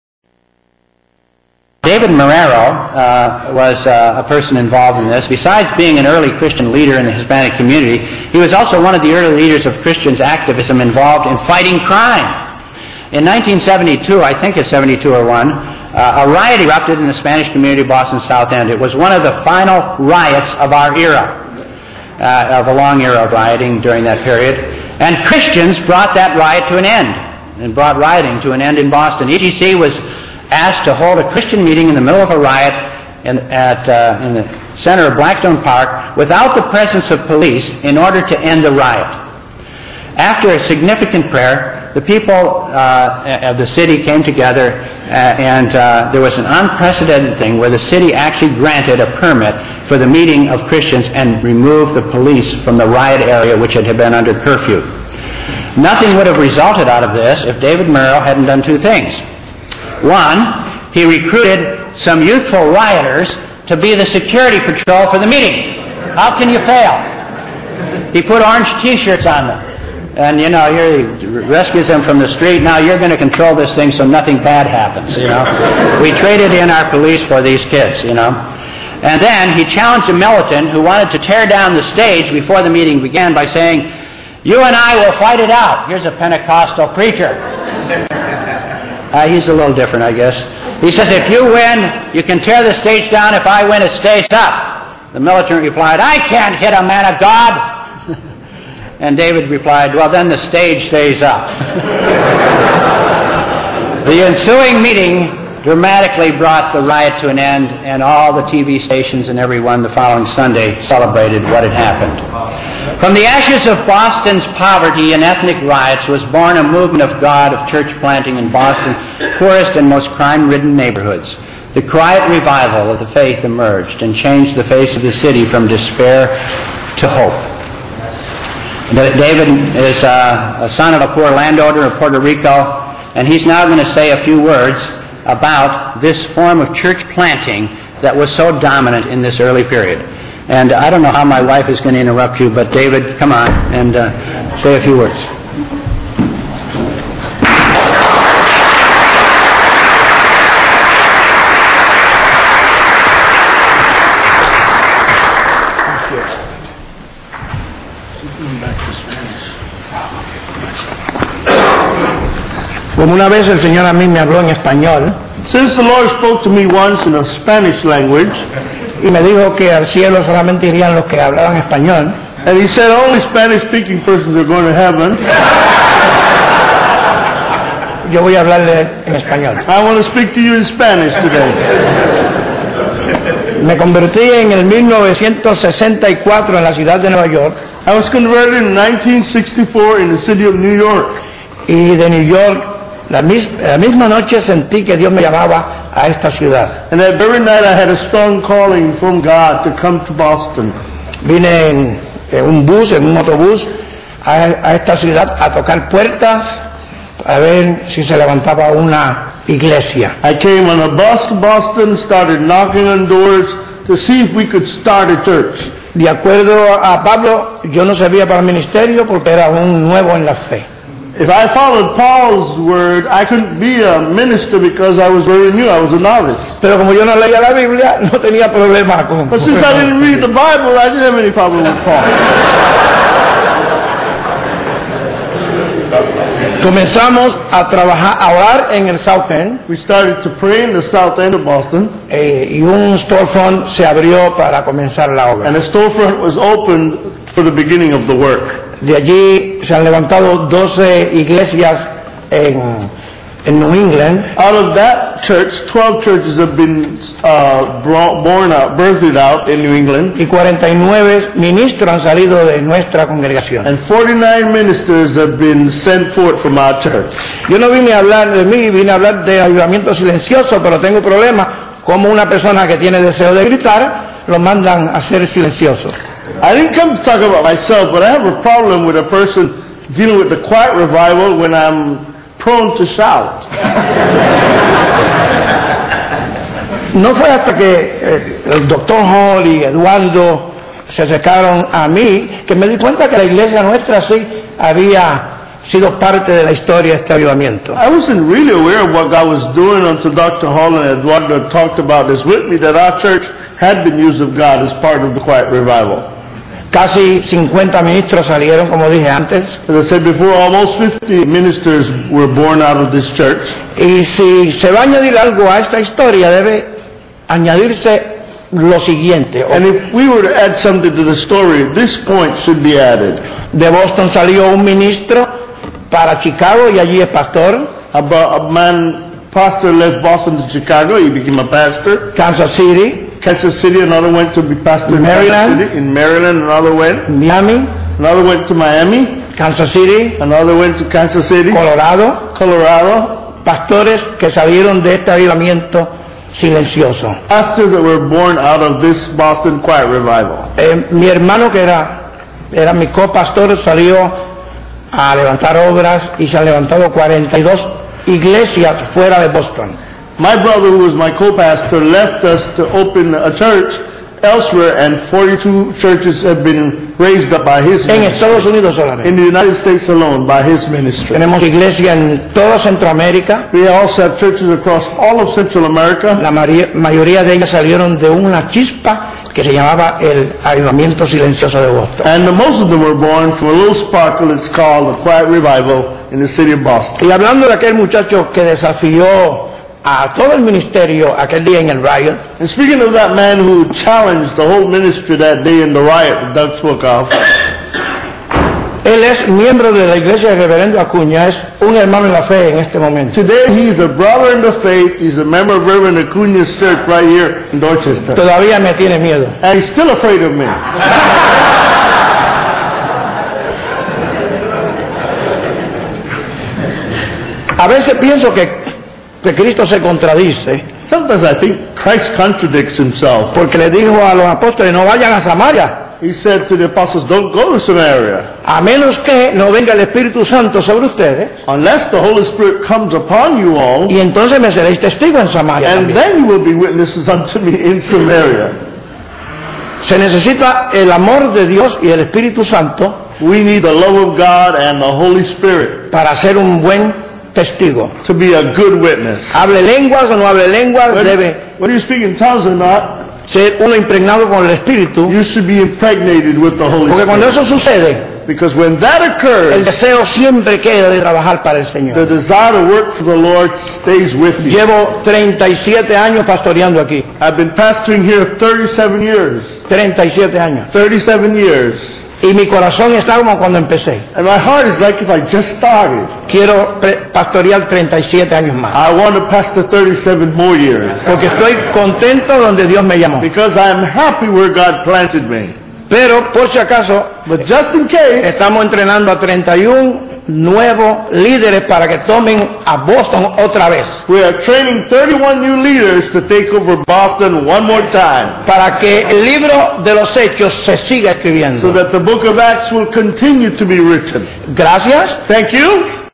David Marrero Sermon: EGC Intercultural Ministries Confernce 2002
Emmanuel Gospel Center's Intercultural Ministries Conference in 2002 has guest speaker Christian activists and leader in the Hispanic community, David Marrero.
Get more free courses for Christian nonprofits and urban ministries